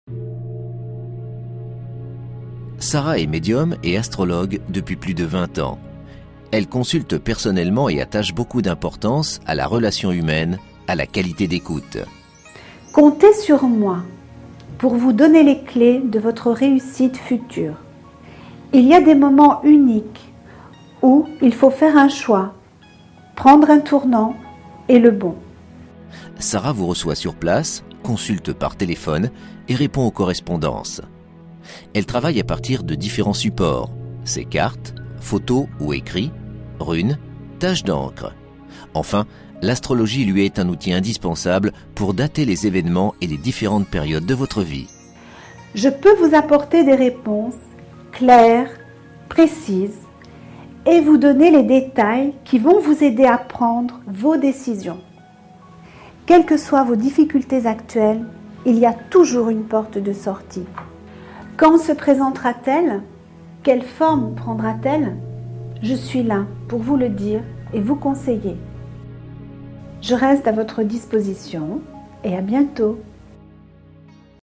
Sarah est médium et astrologue depuis 20 ans à Paris 75003
Sarah se tient à votre disposition pour des consultations privées en cabinet, au téléphone ou par correspondance, elle dévoile votre avenir, répond à toutes vos questions Sarah